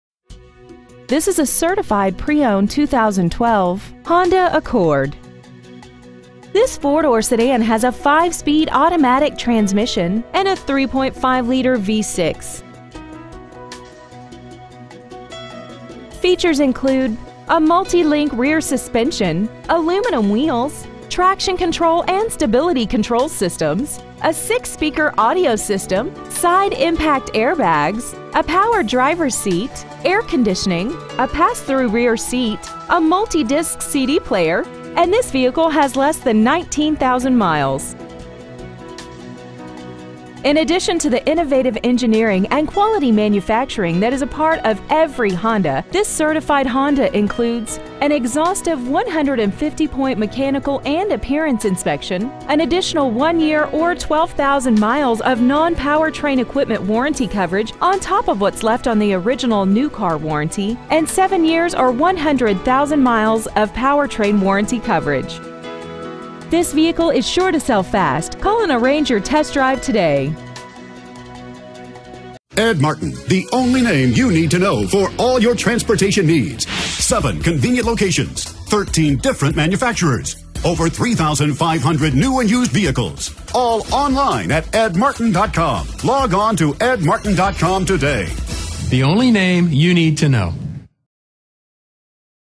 2012 Honda Accord Sdn Certified Indianapolis IN 46219
We've been honored to serve the Indianapolis IN area , we promise that your experience at our dealership will exceed your expectations ! Year : 2012 Make : Honda Model : Accord Sdn Engine : Gas V6 3.5L/212 Trans . : Automatic Exterior : White Orchid Pearl Miles : 18,233 Stock : 1P5668 Ed Martin Nissan North Shadeland Ave Indianapolis , IN 46219